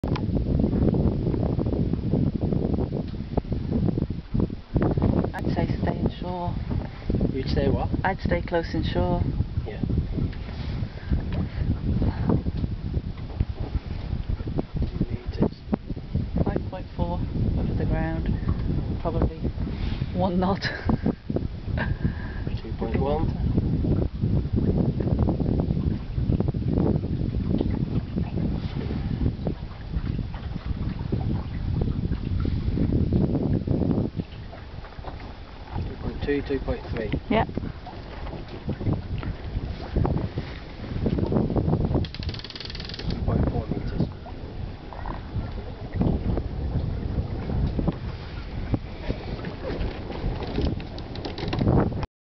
Sailing Morecambe Bay to Arnside 3
Part 3
Sounding our way up the Kent channel on the flood tide with only 2 metres depth of water!